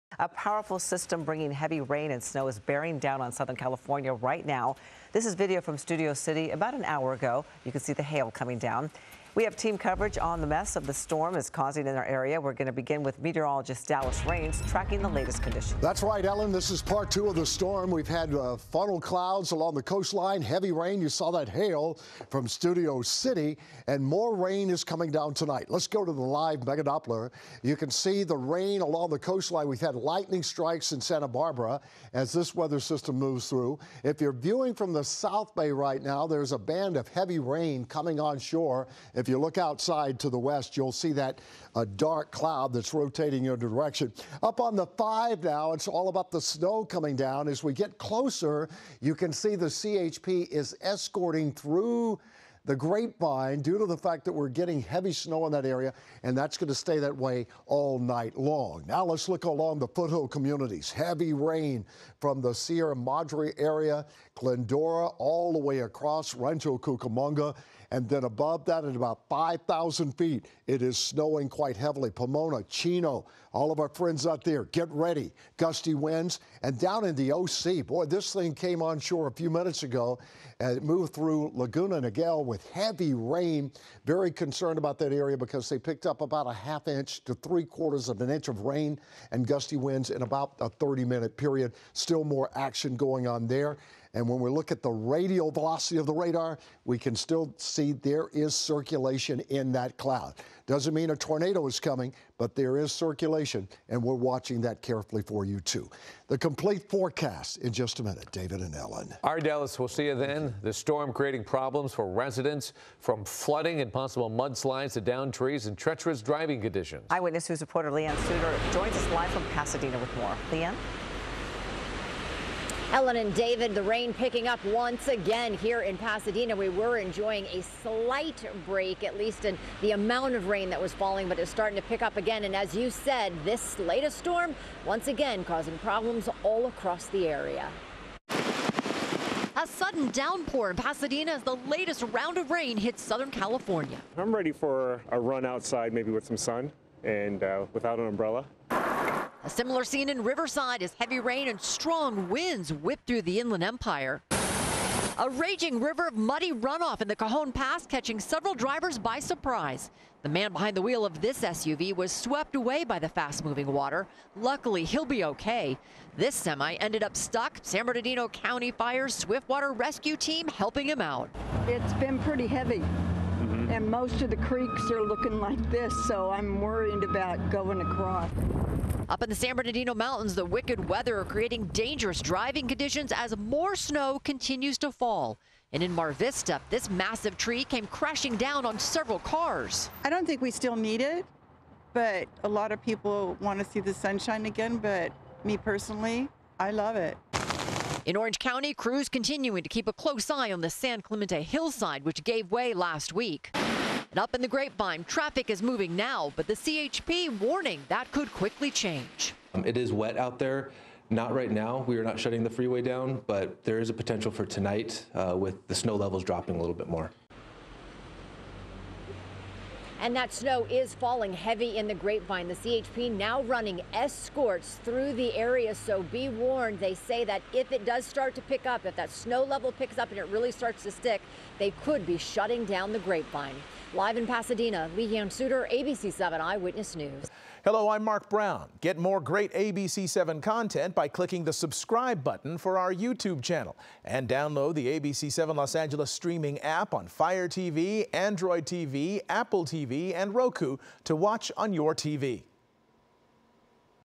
Hail spotted in Studio City as more wind, rain and snow soaks SoCal
A strong late-season Pacific storm bringing heaving rain and snow hit Southern California hard on Tuesday and it's expected to continue for another day.